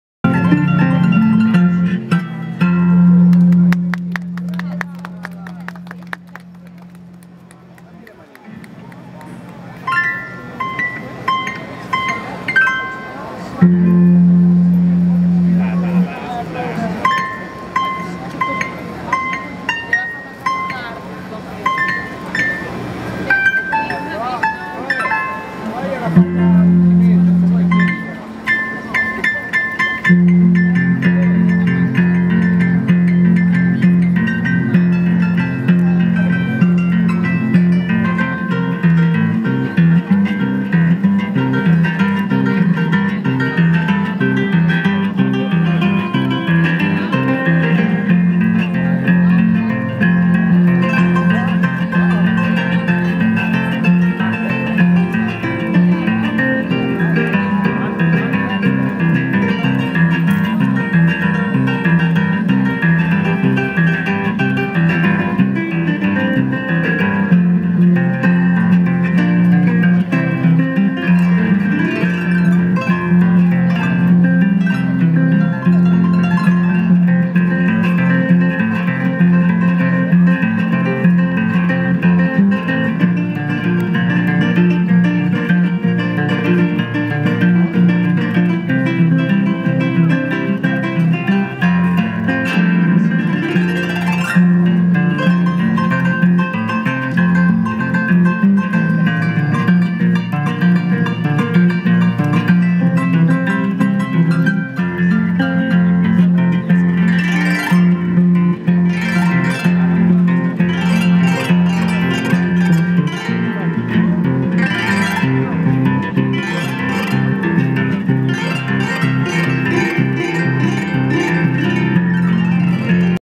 Gademusikanter, Rom 2015
Gademusikant på Piazza Navoni, Rom
22. april 2015